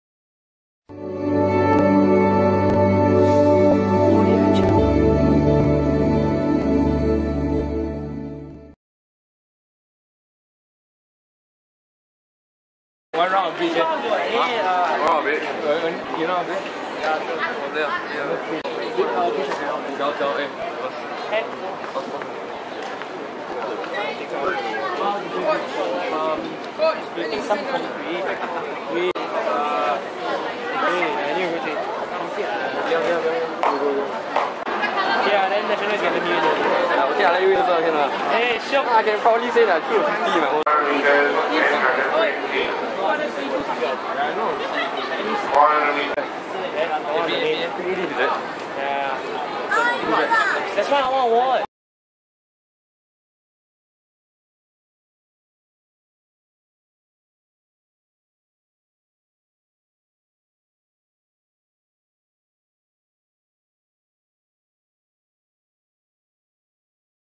Shot Put Girls U18 3kg - 2012 SAA T&F Series 3
Date: Saturday & Sunday, 3 and 4 March 2012

Venue: Bishan Stadium

Not all events/attempts were recorded.